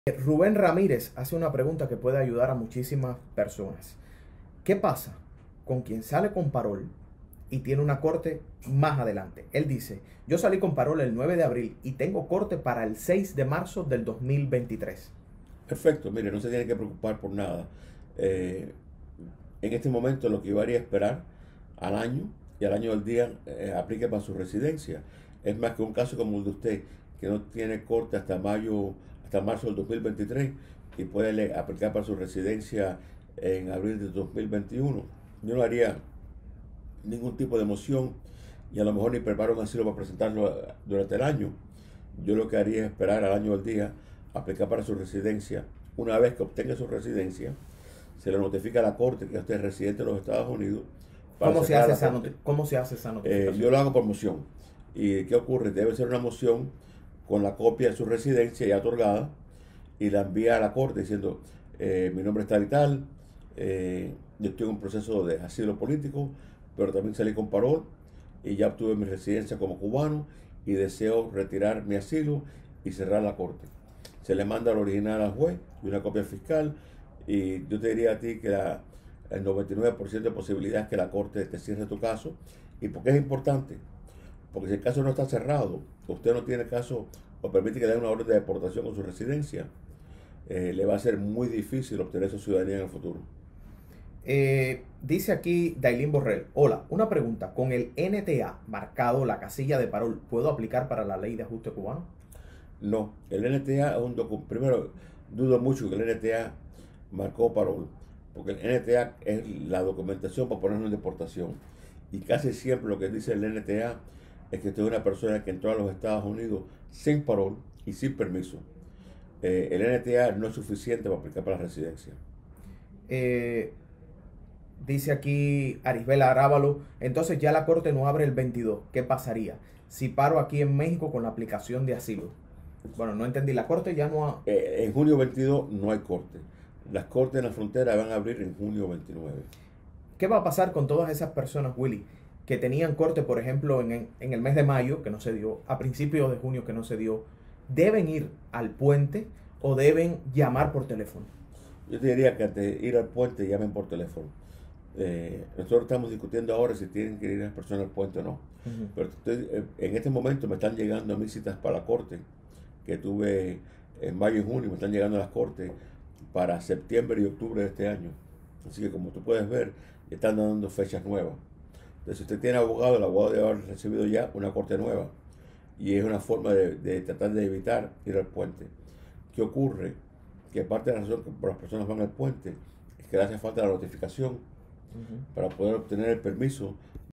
EXPLICACIÓN SOBRE PAROLE Y LA FECHA PARA APLICAR A LA RESIDENCIA PERMANENTE BAJO EL AJUSTE CUBANO
PARA MUCHOS MIGRANTES CUBANOS EL PAROLE SE CONVIERTE EN LA ENTRADA SOÑADA AL TERRITORIO DE LOS ESTADOS UNIDOS PORQUE LES PERMITE LEGALIZARSE AL AÑO Y UN DÍA. ENTRE LAS PREGUNTAS MÁS HABITUALES ESTÁN CUÁNDO APLICAR A LA RESIDENCIA, SI CUANDO SE ENTRÓ A LOS ESTADOS UNIDOS O POR LA FECHA DEL PAROLE. EL ABOGADO DE INMIGRACIÓN WILFREDO ALLEN ACONSEJA HACERLO POR LA FECHA DEL PAROLE.